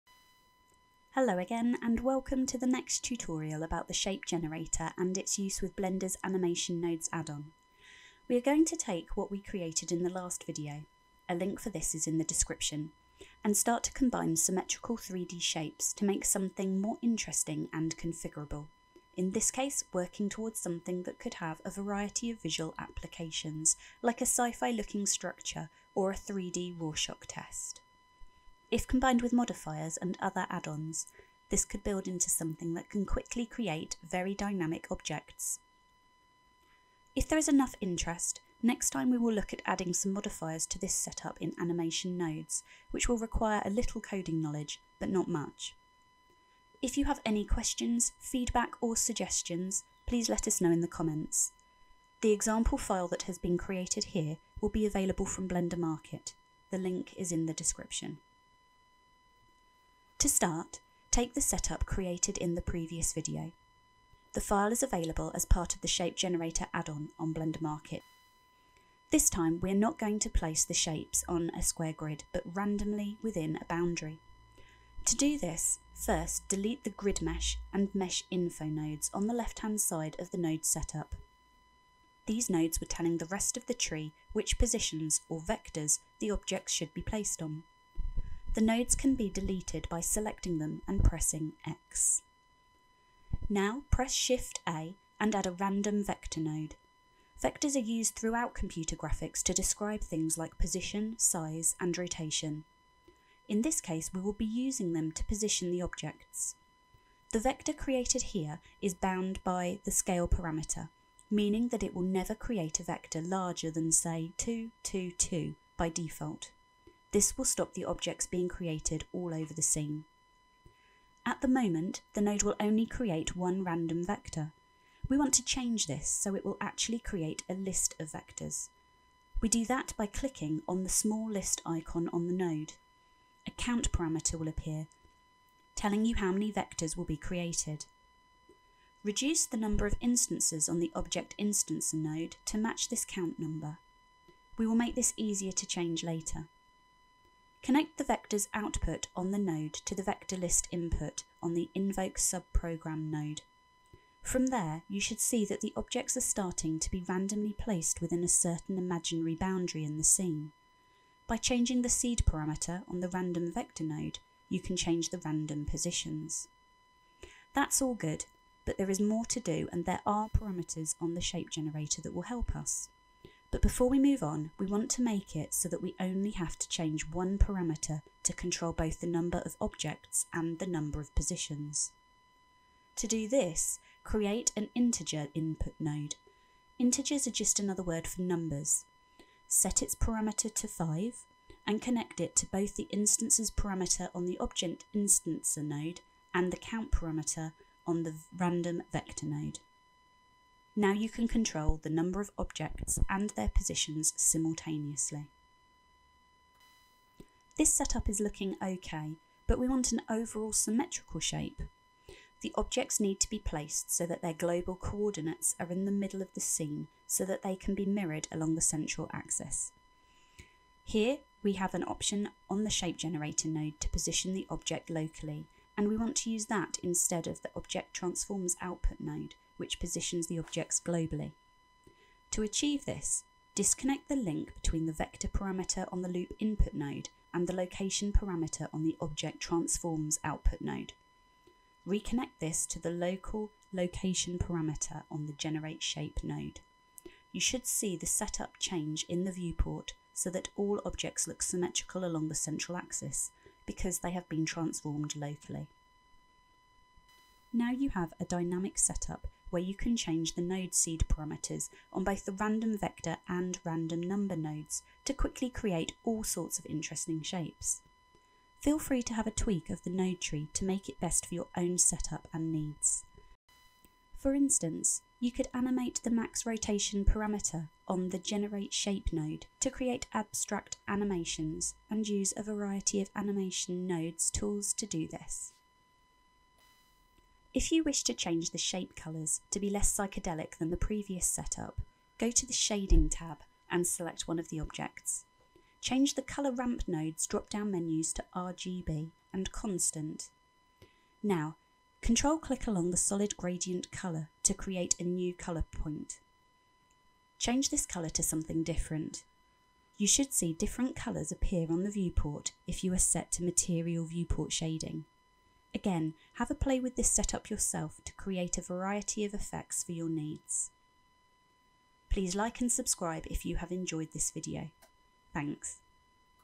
3. Combine 3D shapes with the Shape Generator and Animation Nodes
Next tutorial on the Shape Generator's animation nodes feature: Combining symmetrical 3D shapes to make something more interesting and configurable, in this case working towards something that could have a variety of visual applications, like a Sci-Fi looking structure or a 3D Rorschach ("ROR-shock") test.